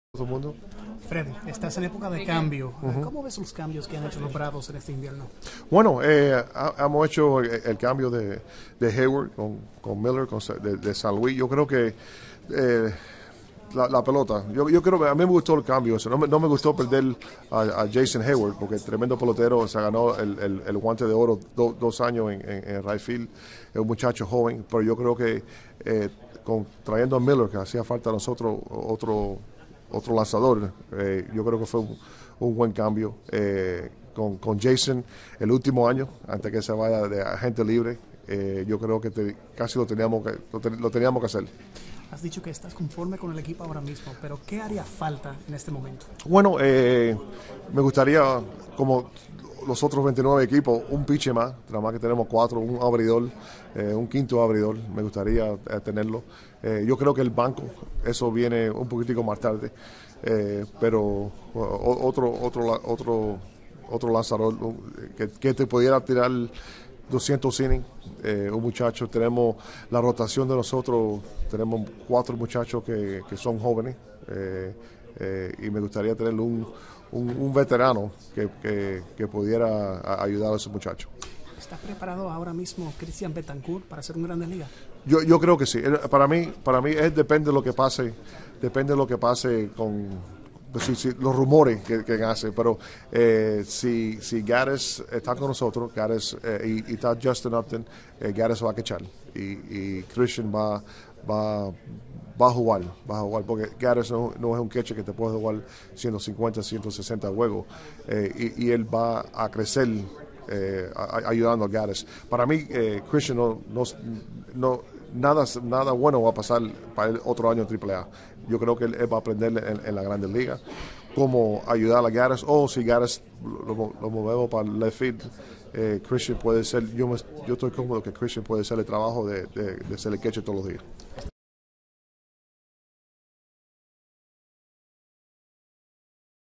Fredi sobre sus Bravos
Fredi Gonzalez comenta sobre el progreso de los Bravos y sus movimientos en este invierno